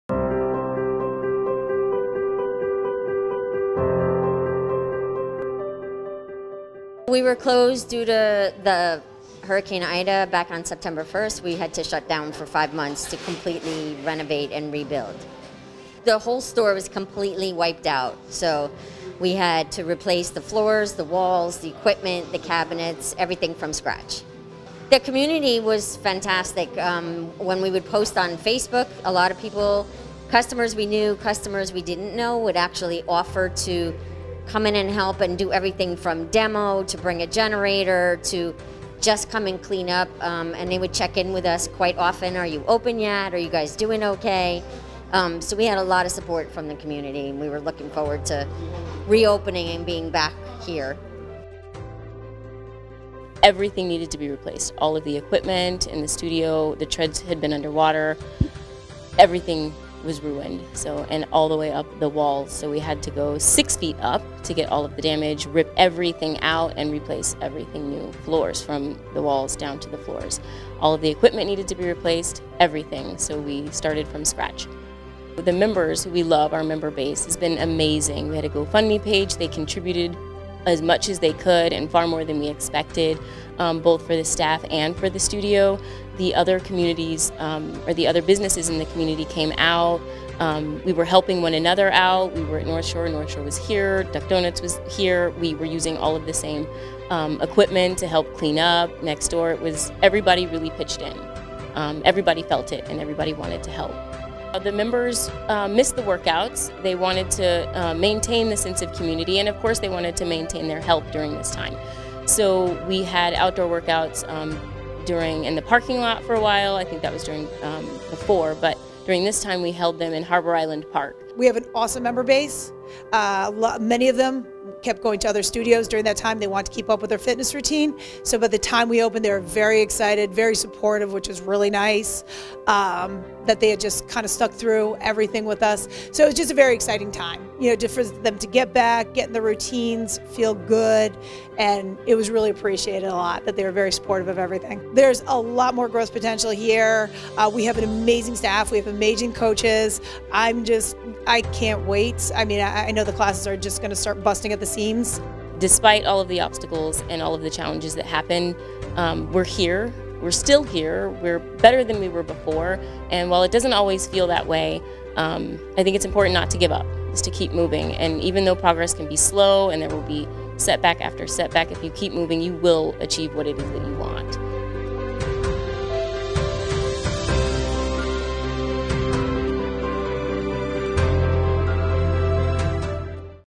Mamaroneck Businesses Reopen
Check out the Mamaroneck business that have re-opened after the devastation from Hurricane Ida in September.  We speak with some local businesses Duck Donuts and Orange theory, but located in the same shopping center on Mamaroneck Ave,  who recently reopened their doors.